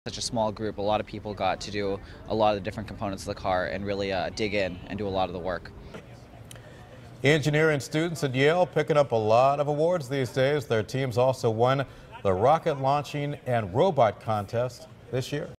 Yale racecar wins international competition
A fuel-efficient car built by Yale students